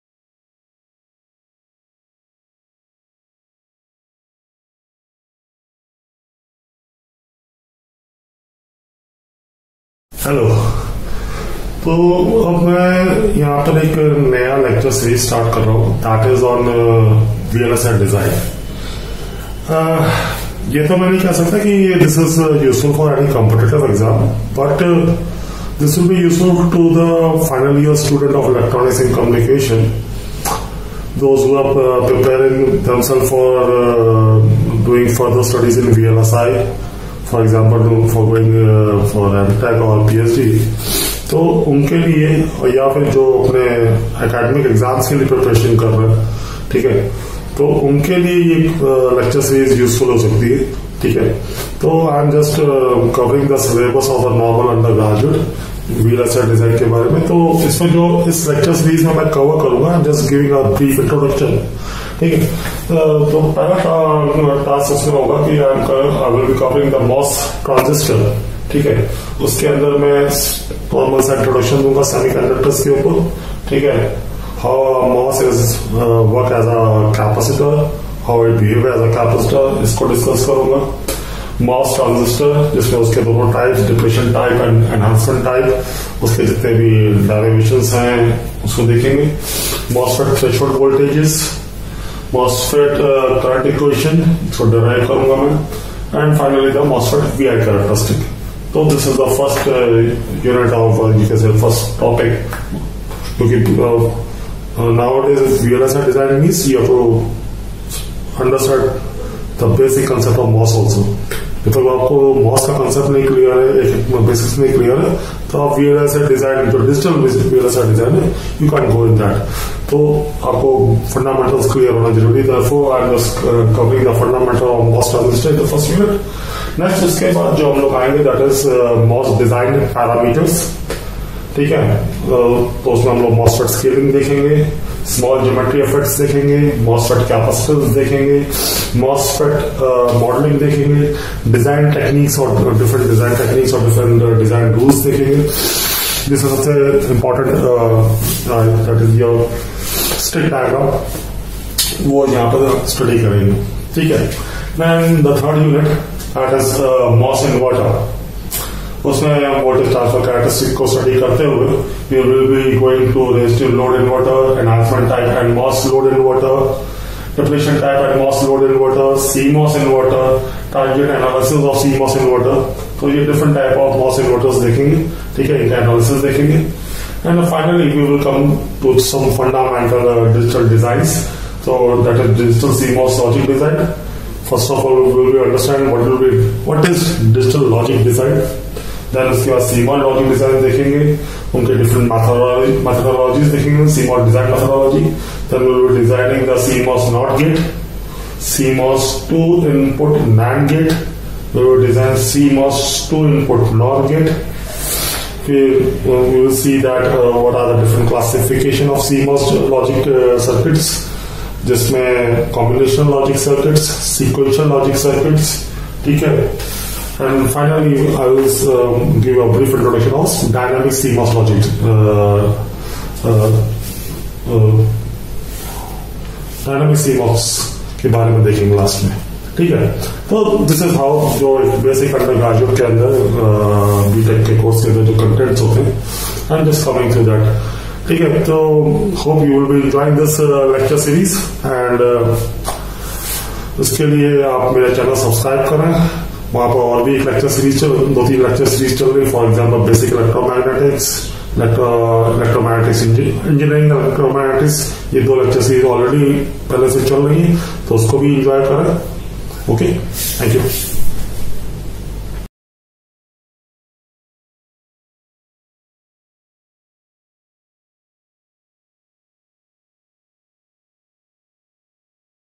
Introduction to VLSI DESIGN Lecture series
VLSI Design Lecture series.Here I have given brief outline of the VLSI design Lecture series.Topics and content in The VLSI design Lecture series.